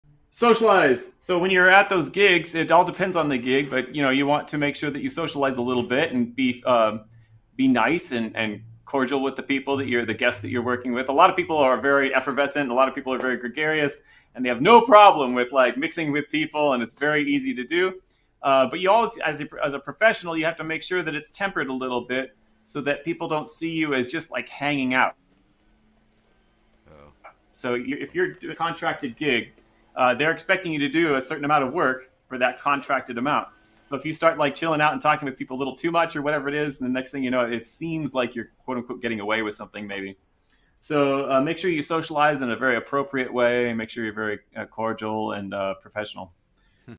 Uncovering the Professional Secrets of Socializing as a Musician
Socialize! So when you're at those gigs, it all depends on the gig, but you want to make sure that you socialize a little bit and be nice and cordial with the people who you're the guest that you're working with. A lot of people are very effervescent. A lot of people are very gregarious and they have no problem with mixing with people and it's very easy to do. But you always as a professional, you have to make sure that it's tempered a little bit so that people don't see you as just like hanging out.